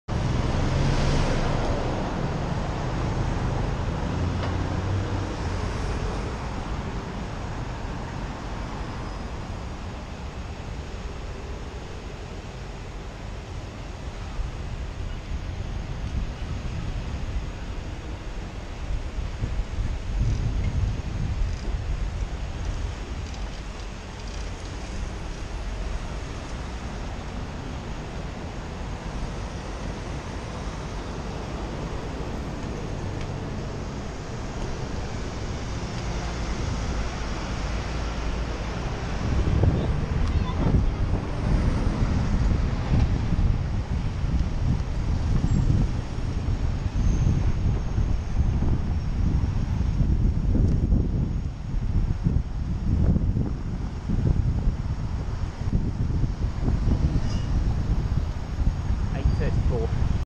WB56MRB MRB Transport Illegal left turn
HGV WB56MRB from MRB TRansport making an Illegal left turn through the cycle lane at Victoria Embankment / Westminster Bridge junction.